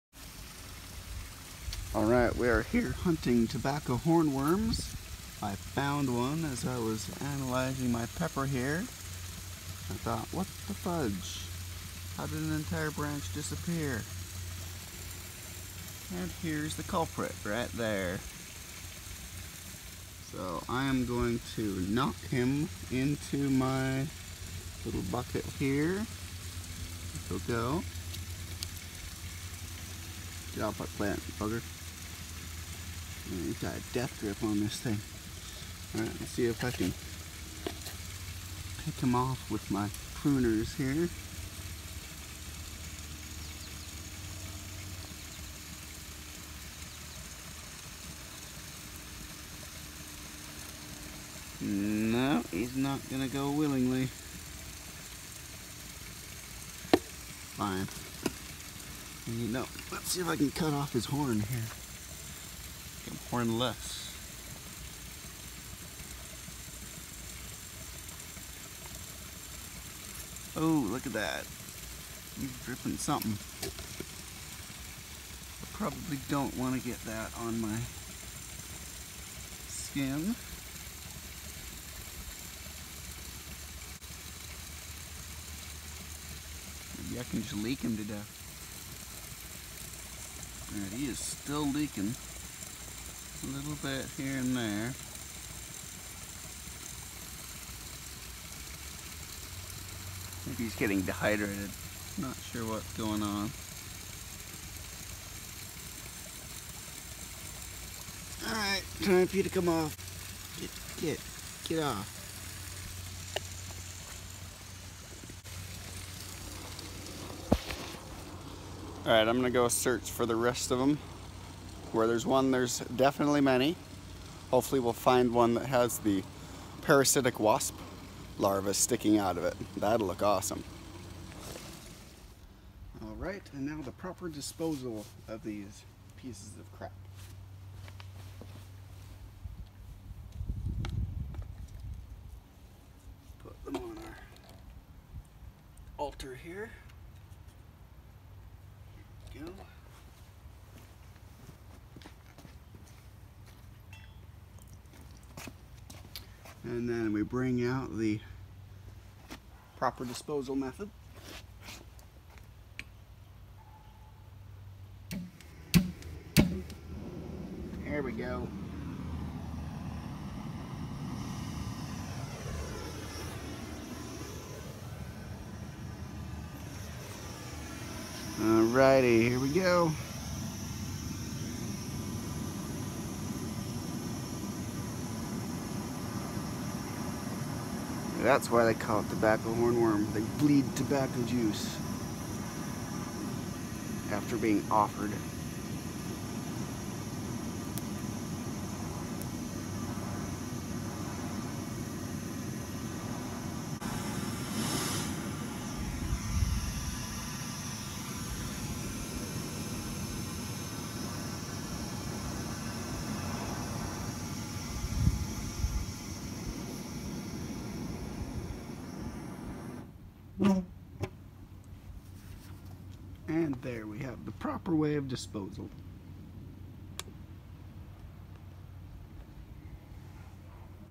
Sacrificing hornworms 😈
My first encounter with hornworms, the bane of all gardeners everywhere. So I did the right thing and made a burnt offering to the garden gods ;)

Hornworms are the caterpillar stage of the sphinx moth. These crawlers can defoliate a whole plant in under a day and a severe infestation can decimate an entire farm in no time.

The 3 I found today are just babies; they’re known for reaching 4 inches long by the time they’re ready to transform.

Control methods include:
Diatomaceous Earth, a white powder of the crushed remains of fossilized diatoms, a single celled algae made of silica. Imagine being covered in broken glass shards from lightbulbs, that’s what it does to soft-bodied insects.
Bt - a bacterium that produces a certain crystal protein that is toxic to insects.

Both DE and Bt can be used in organic gardening.